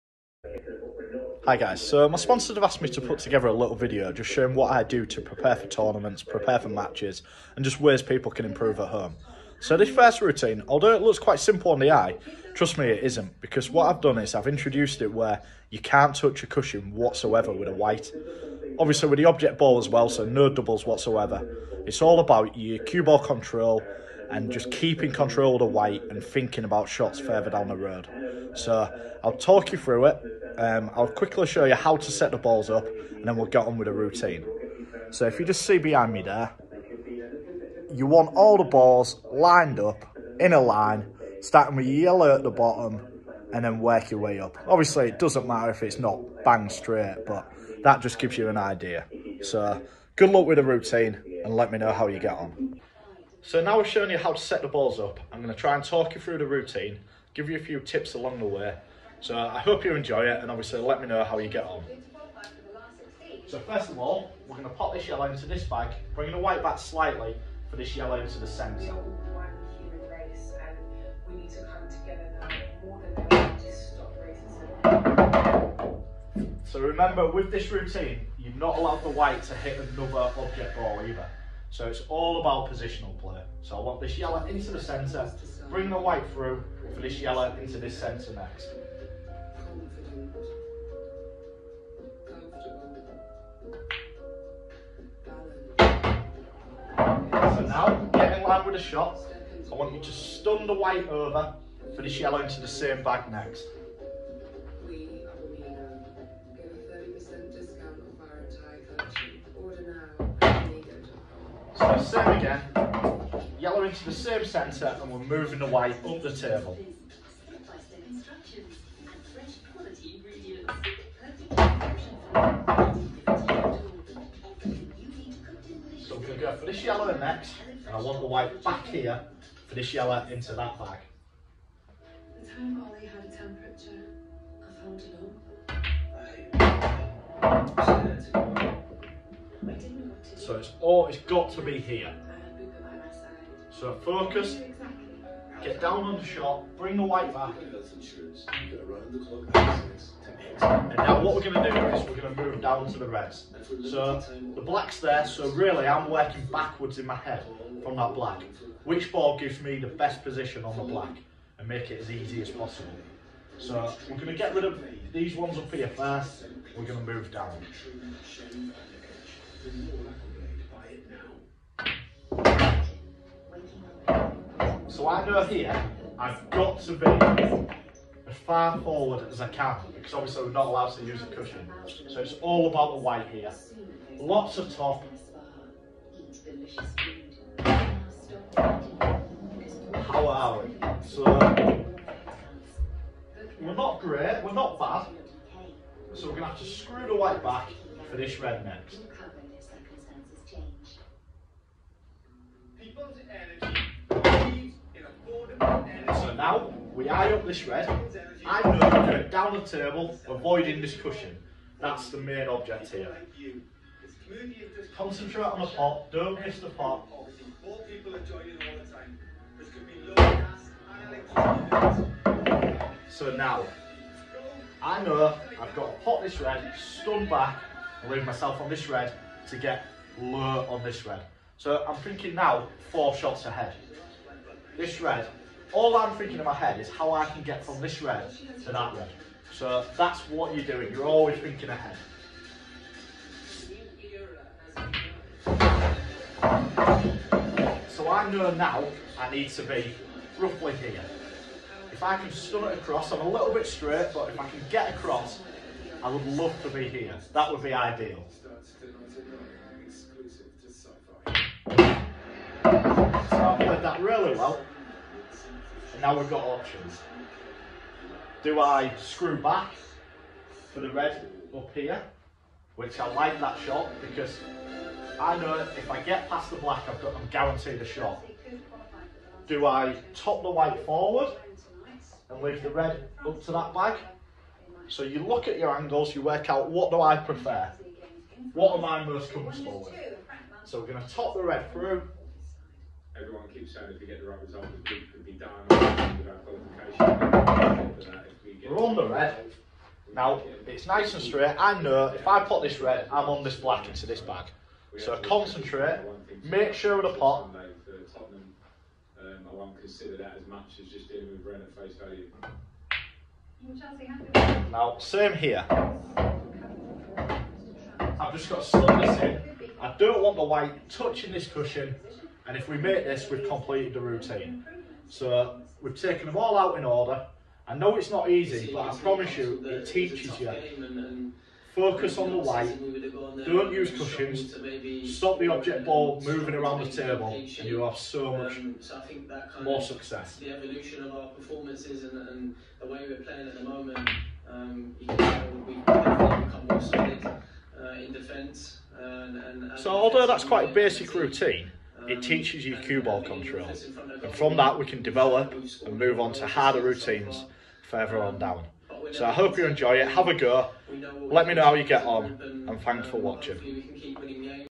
Chris Patrick Pool Coaching - Basic Cue Ball Control Routine
Basic training drill for any newcomer just getting into pool with a few added twists to increase the difficulty level